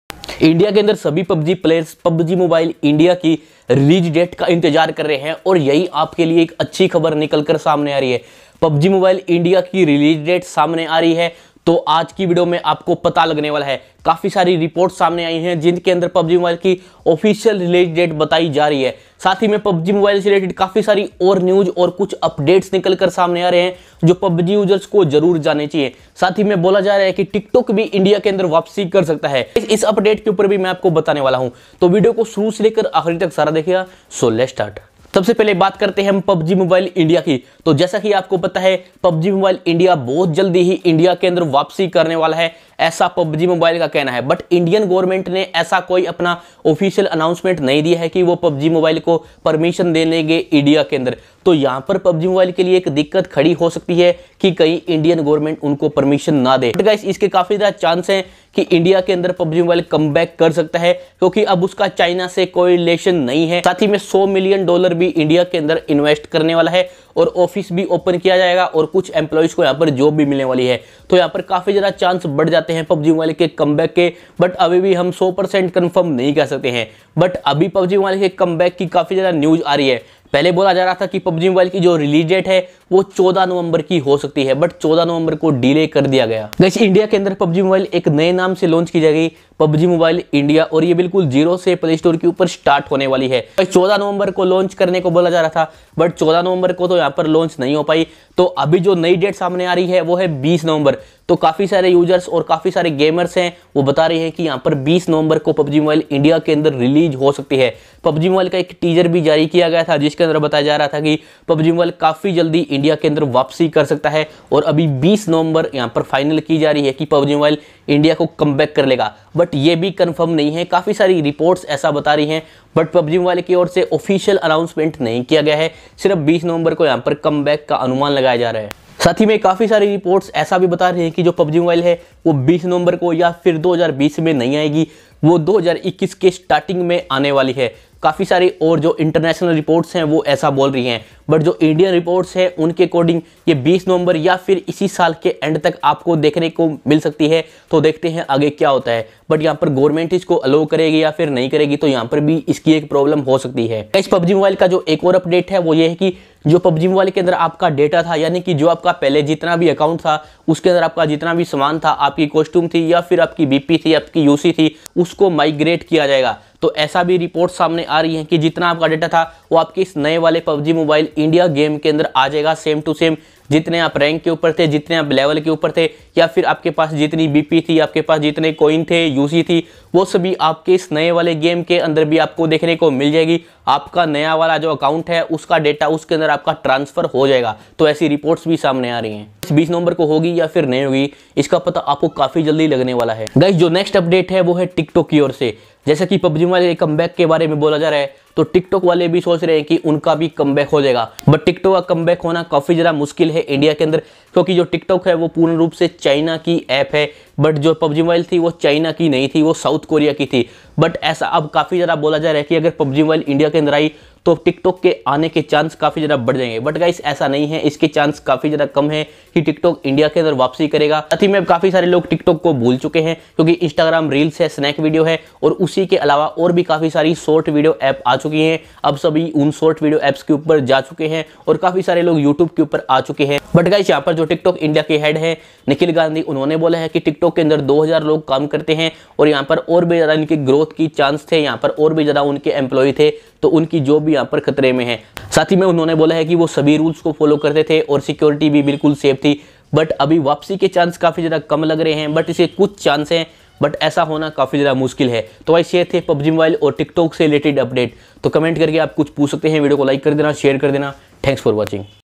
20 November 😱? PUBG Mobile India Release Date | TikTok Comeback Plan In India? Big News
PUBG Mobile Release Date Coming In India Is 20 November 2020, According to some media reports. So today we are going to tell you about, PUBG Mobile India launch date and other some updates. Tik Tok also planning to come back in India like PUBG. I hope you will like this video.

About This Channel:
Tech Takneek Is A Technical YouTube Channel. Which Are Providing Tech & Gadgets Related Content Daily.

Jai Hind 🇮🇳